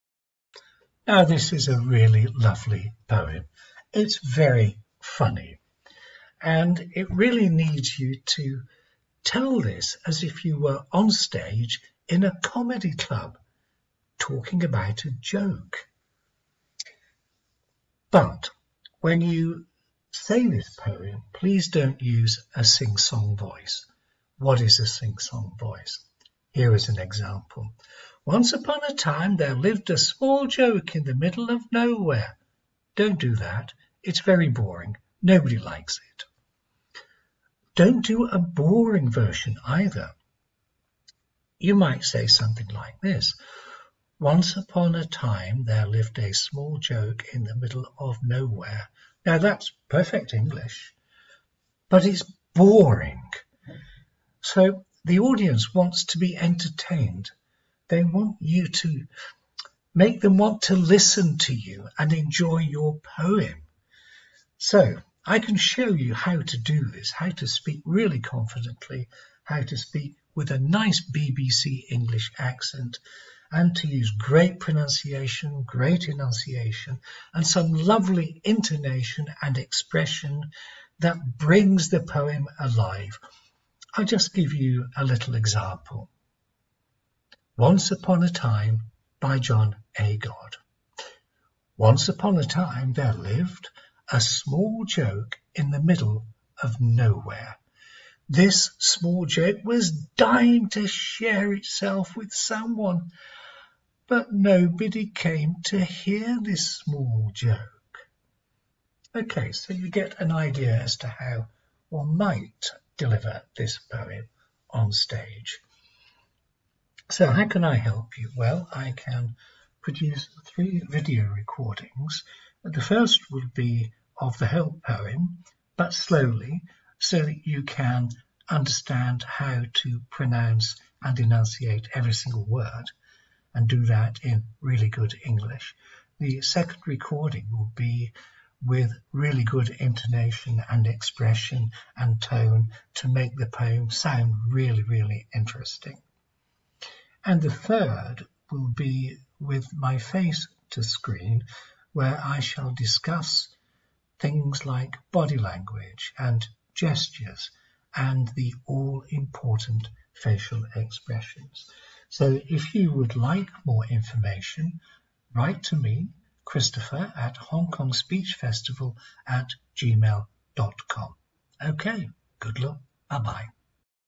Once Upon a Time, by John Agard: How to perform your poem with confidence and in perfect BBC English
Let me record your chosen poem for you so that you can speak it in perfect English and perform it in the most attractive way so as to attract your audience.